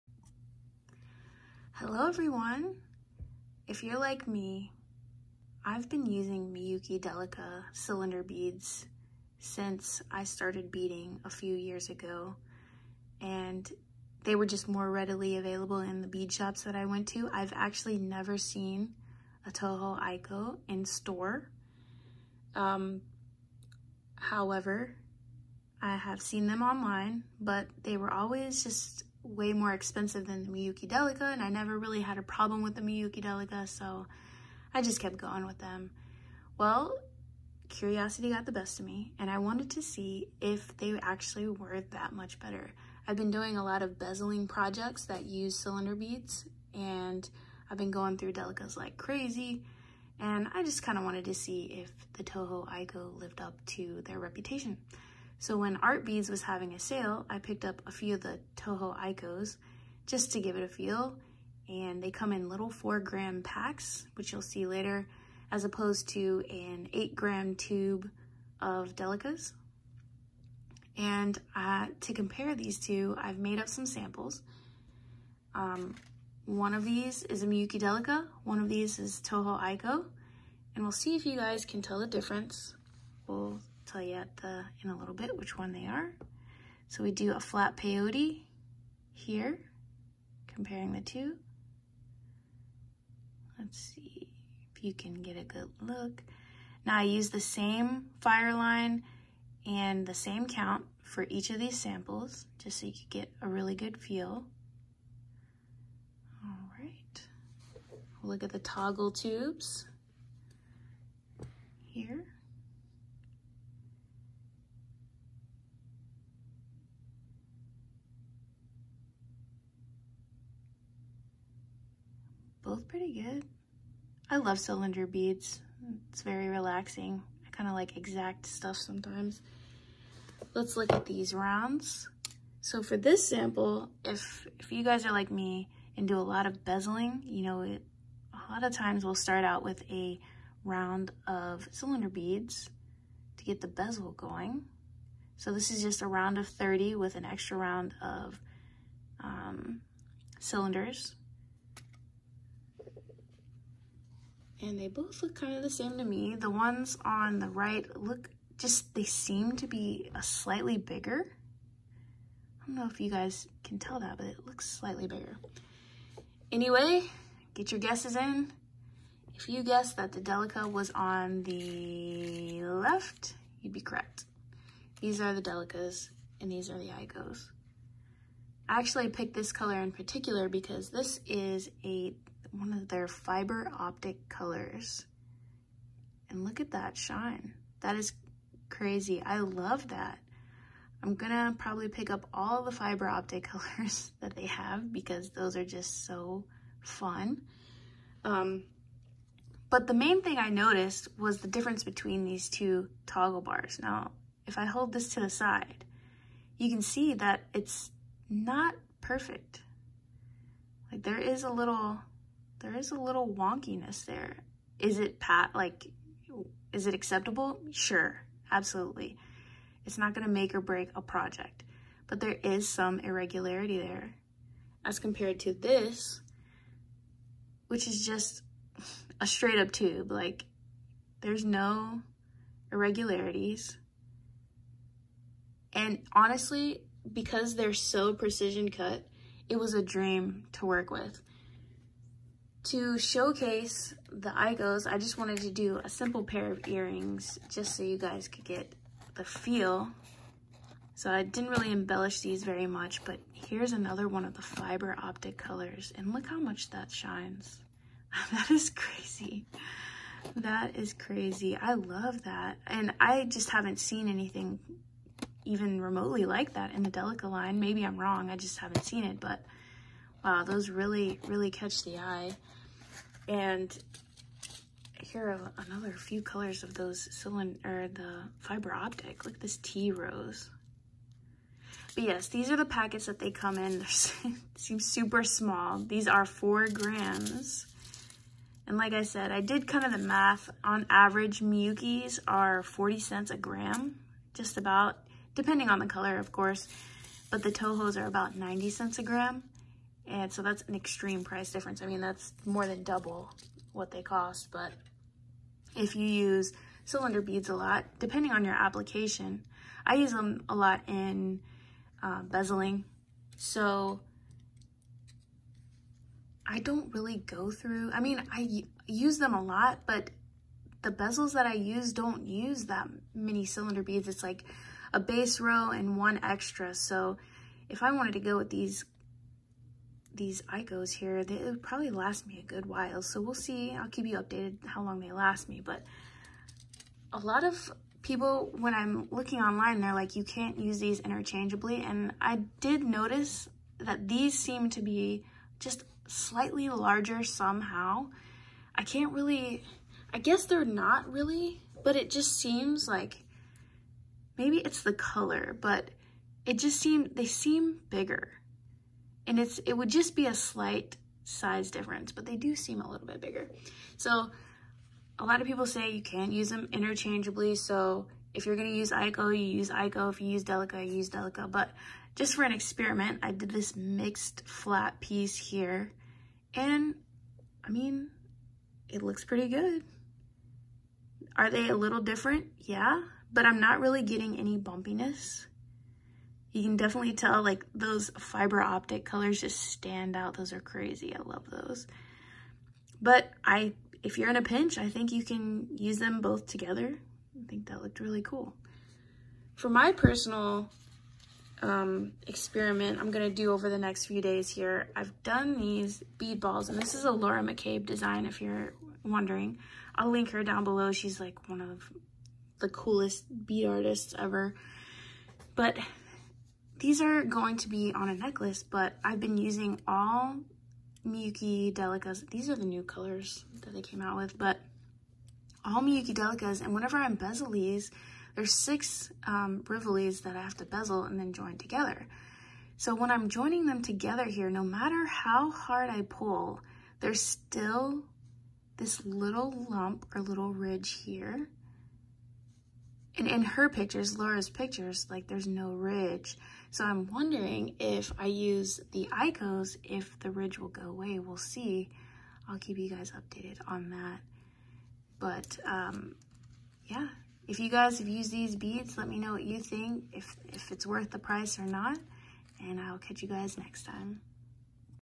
Toho Aiko vs. Miyuki Delica: Worth the price?
Hello beaders!✌🏻

Today we take a look at the different between Miyuki Delica and Toho Aiko beads. Have you tried them? What are your thoughts? Let me know!

If you would like to share your projects with me, please tag me on Facebook or Instagram!

Happy beading!

Kirk